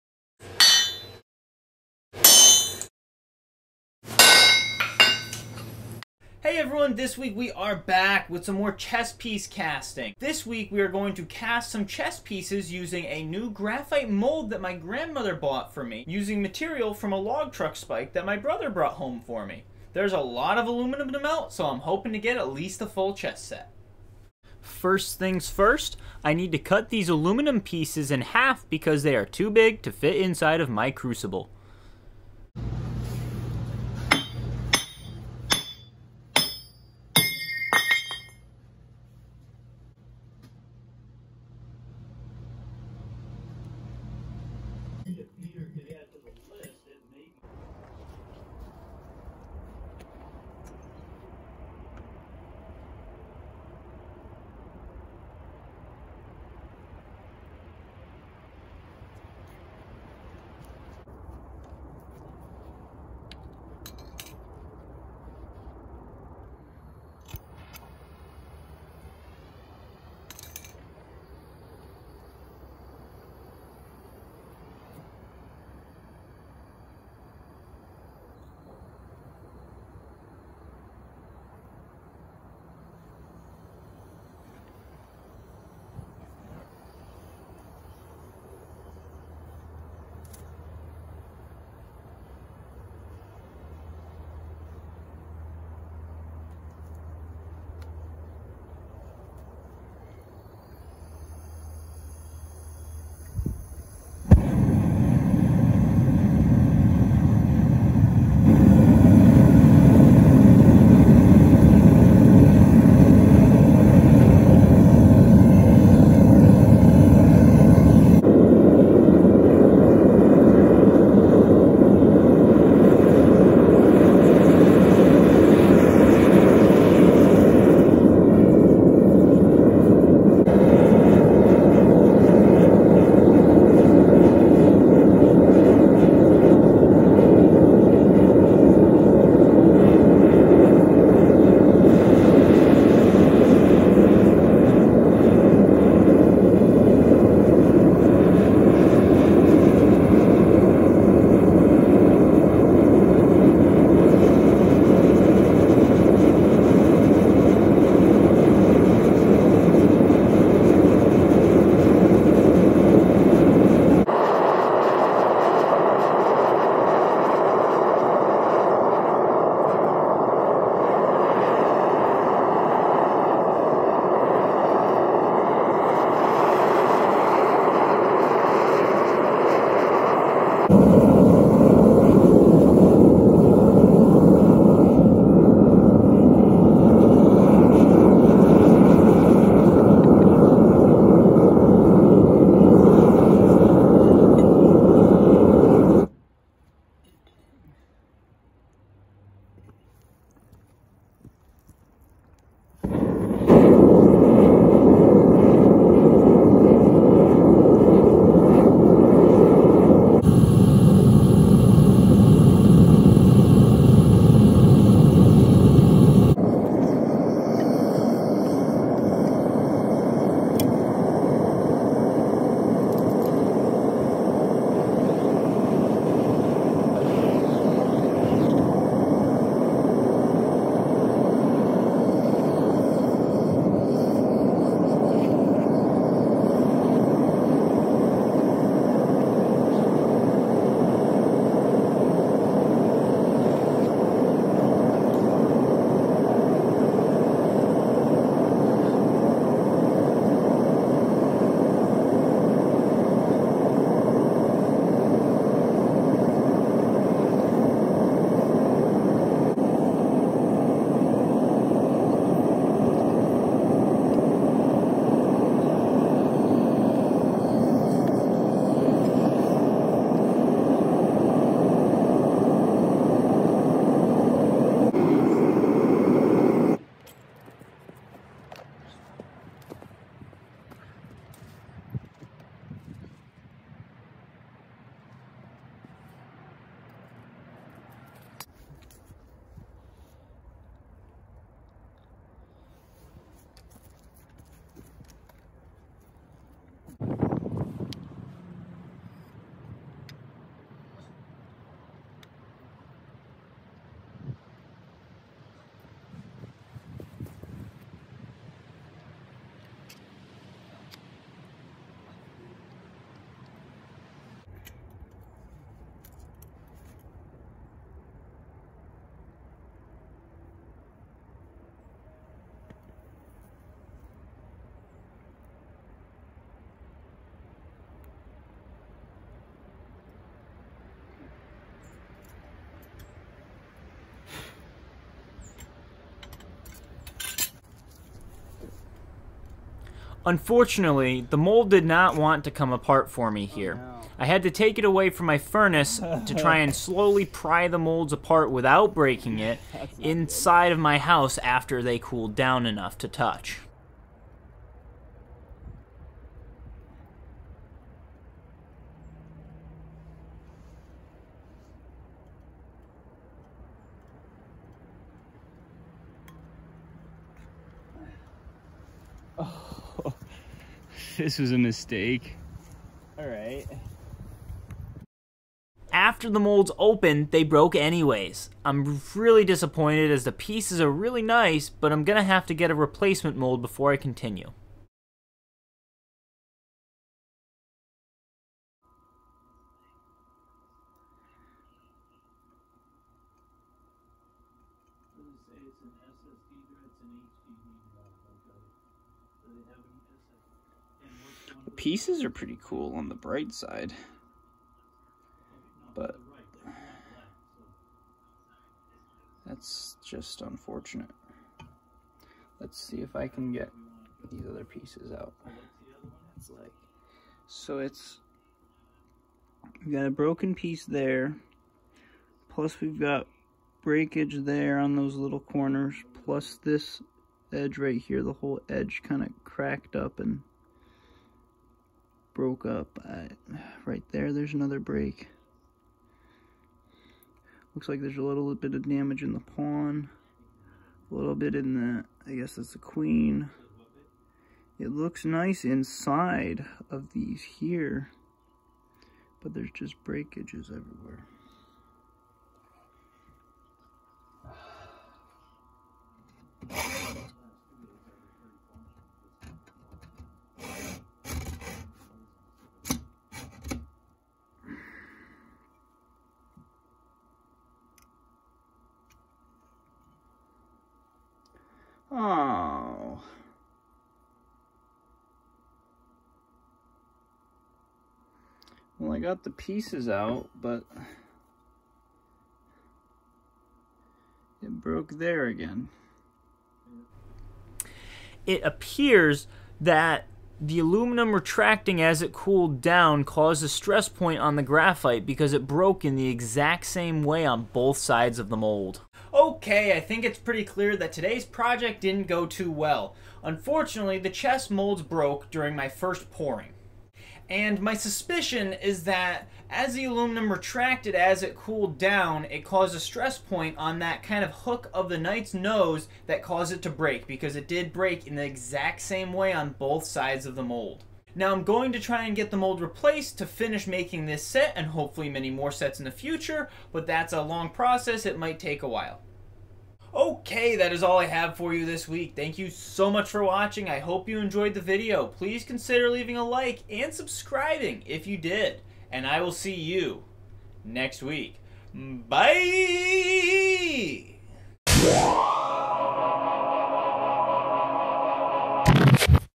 Aluminum Chess Piece Casting [Fail] | My New Mold Broke!
Hey everyone! This week I am back with another chess piece casting video!

I’m going to be using a new graphite chess mold my grandmother bought for me to make the chess pieces.

Unfortunately this did not go to plan and my mold broke on the first pour, however I did get some cool pieces out of it! I will be getting this mold replaced to finish the set in the future!

A side note: this is the only time you’re going to see me doing an outro with that high of a pitch.